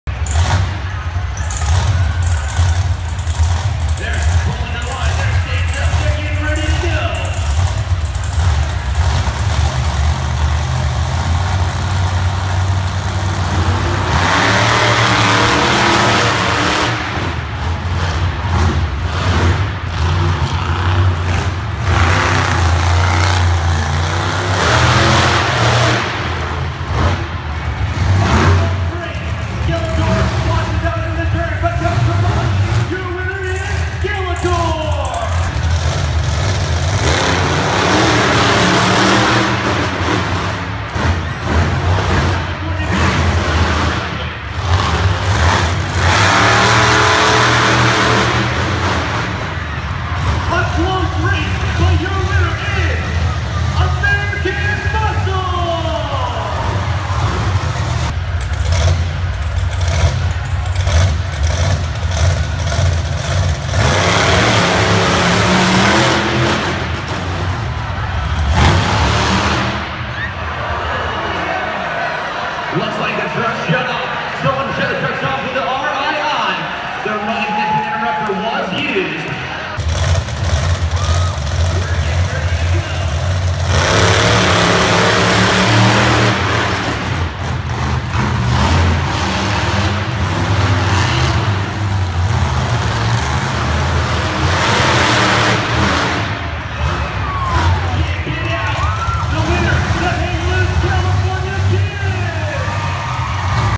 Monster X Redmond, OR Racing 2015
Monster truck racing from the Bank of the Cascades Center in Redmond, Oregon. 3/21/15